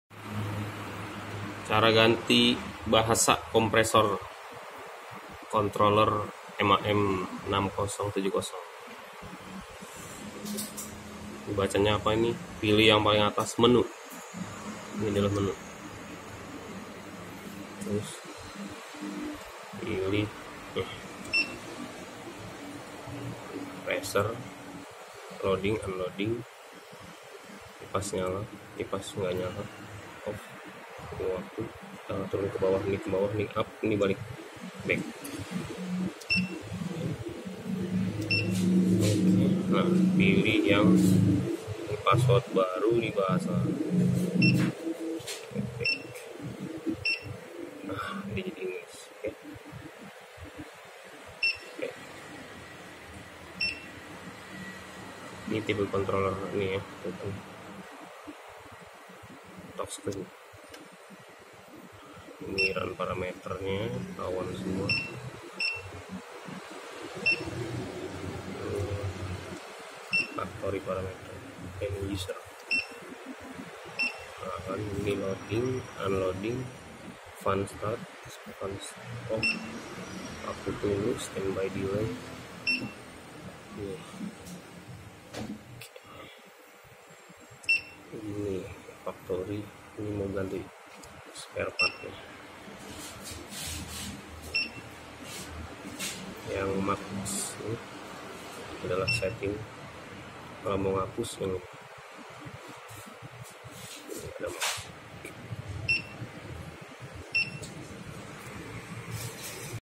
Setting Bahasa English Pada Controller MAM6070 Screw Compressor
Setting Bahasa Chinese - English (Touchscreen)

Sering sekali ditemukan unit Air Compressor di Indonesia beroperasi masih menggunakan Bahasa Chinese, ini dikarenakan pembelian unit mesin produksi dibarengi (include)  dengan unit Air Screw Compressor.

Biasanya pada unit Screw Compressor baru, Preventif Maintenance awal harus pergantian Oli Screw dan Oil Filter 500H, nah kebetulan bahasanya Chinese maka pembeli tidak terlalu faham bahwa adanya lampu Alarm menyala (ada suara teettt) pada layar controller, dan terdapat bacaan Oil Life end (English version), sementara bahasanya masih Chinese pembeli tidak faham dan setelah 100H lewat dari alarm makanl Compressor akan mati tidak bisa di On.
ini berfungsi agar pembeli rutin mengganti Sparepart dan oli Screw agar mesin dalam kondisi Prima.

Kebanyakan pembeli tidak faham karena bahasanya Chinese, maka video ini adalah tutorial cara merubah bahasa Chinese ke English pada Controller MAM6070M Touchsreen, jangan lupa siapkan Password-nya yang diberikan pada saat pembelian mesin.

jika ada Pertanyaan silahkan Hub atau WA di No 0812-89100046

TERIMAKASIH